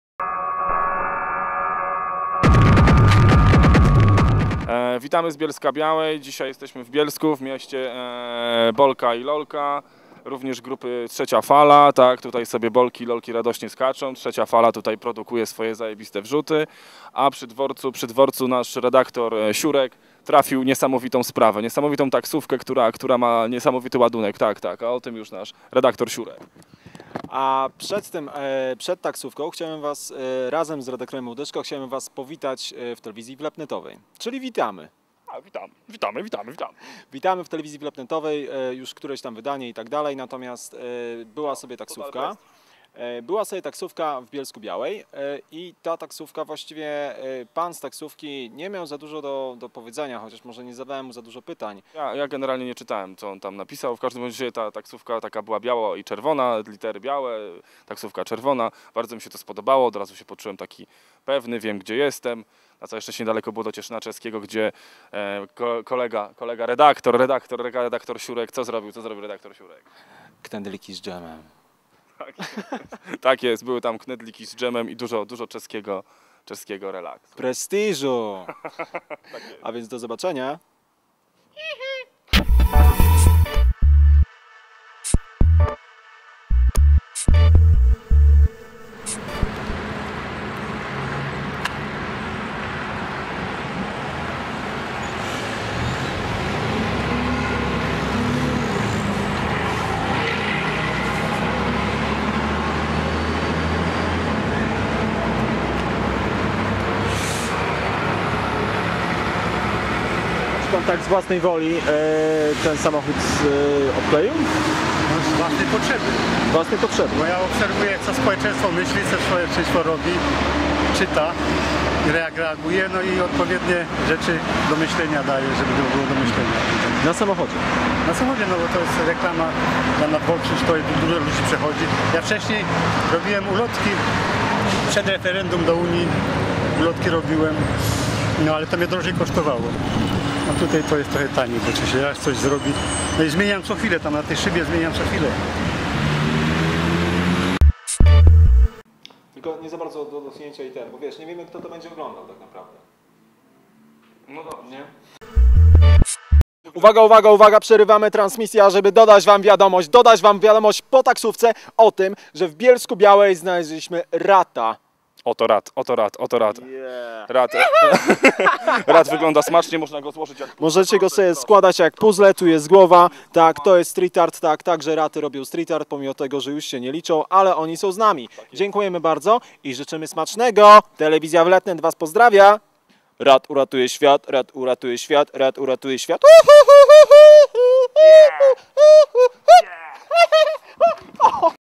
T[V] Super taksówka
Reportaż o super taksówce w Bielsko białej

brygada t[v]